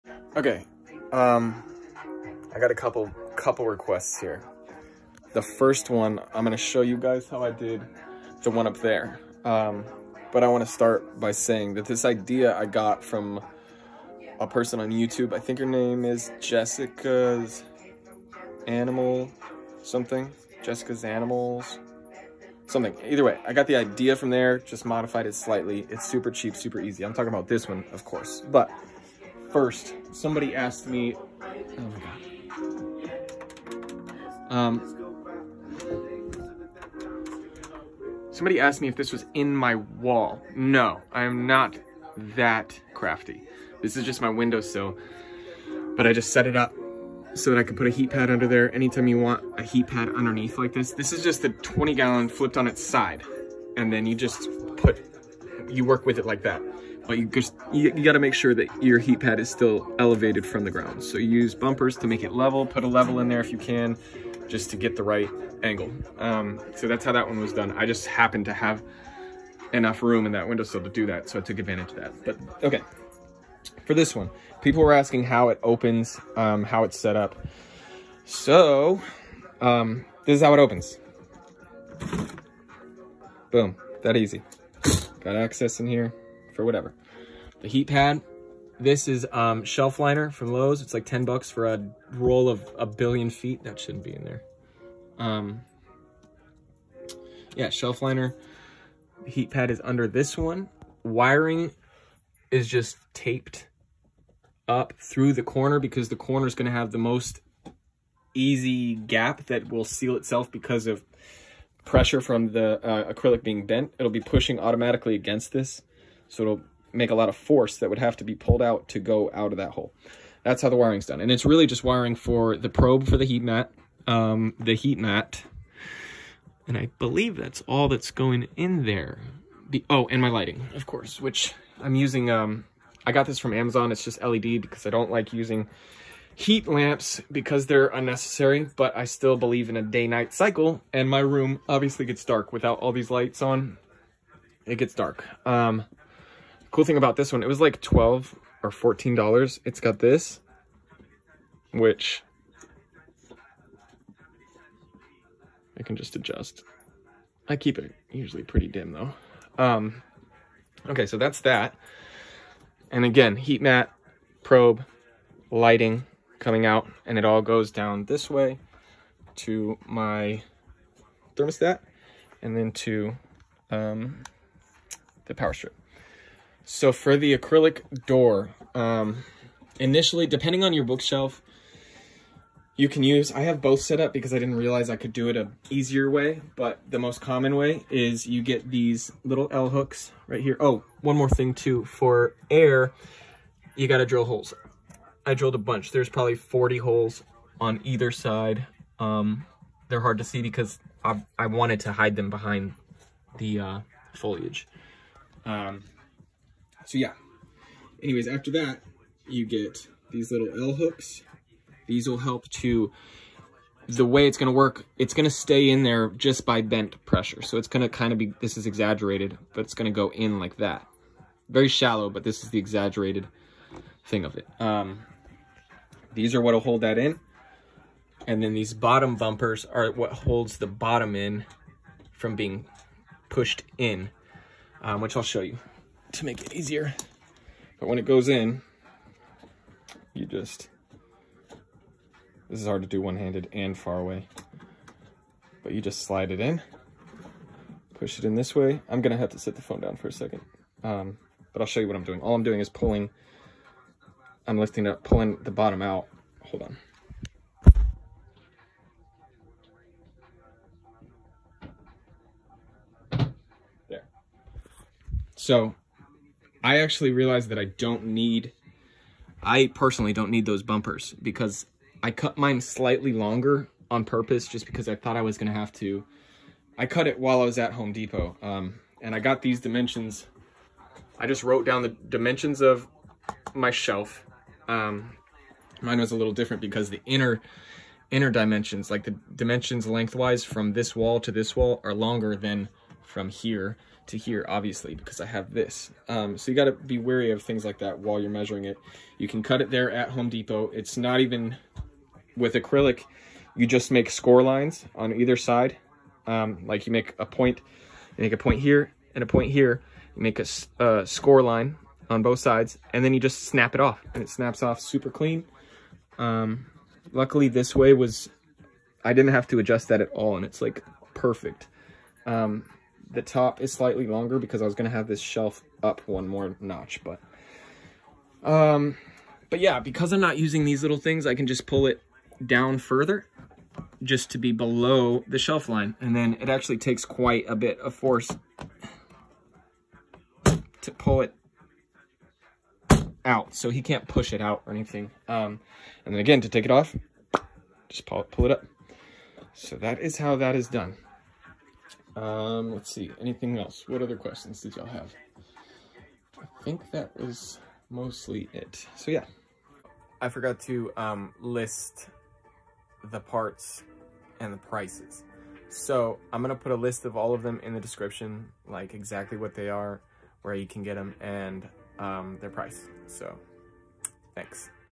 BUDGET Reptile Enclosure Front Opening CHEAP
Components:
Home Depot: 20”x30” acrylic panel - $20, kitchen cabinet liner - $10, command hook (door handle) - $3, “L” screws - $2
Target: Clear Bumpers - $3